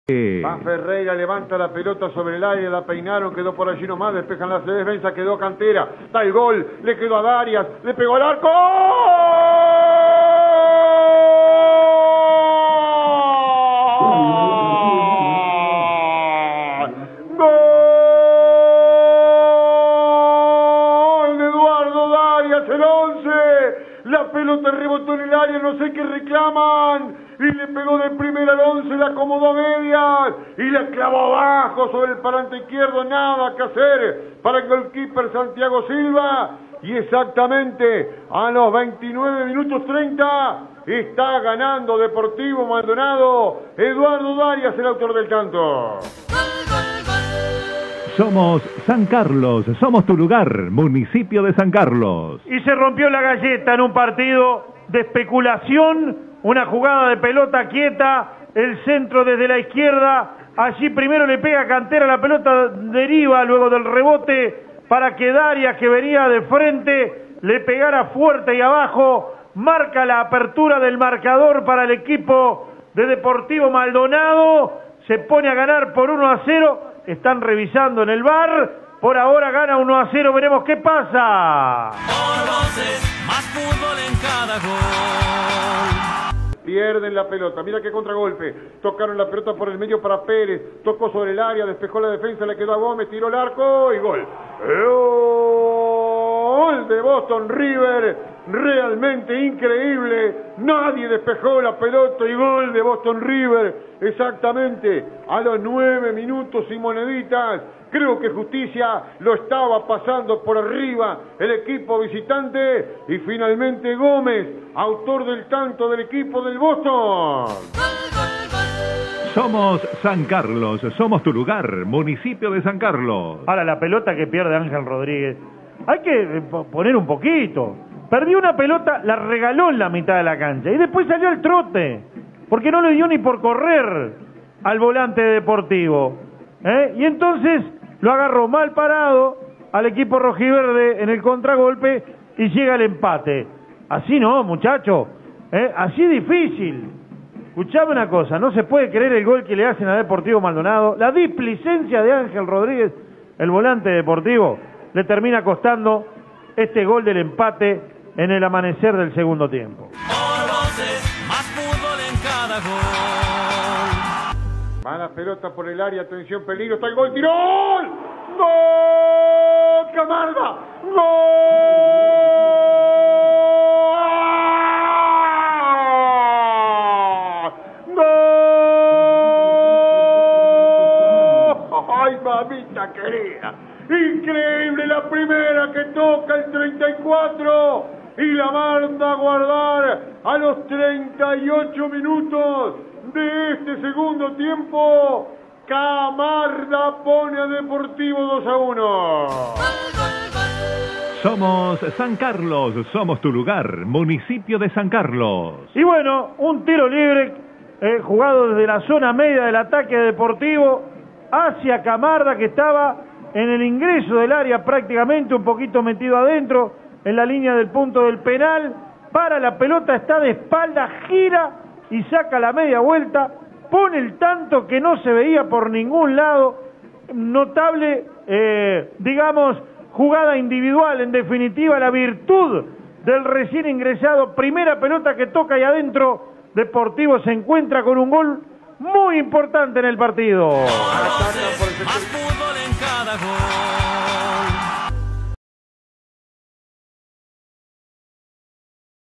Deportivo Maldonado 2 Boston River 1 - Clausura 2022 (por Fernando Mego)
Los goles del partido, por Fernando Mego y Víctor Hugo Castro para FM Voces de Maldonado.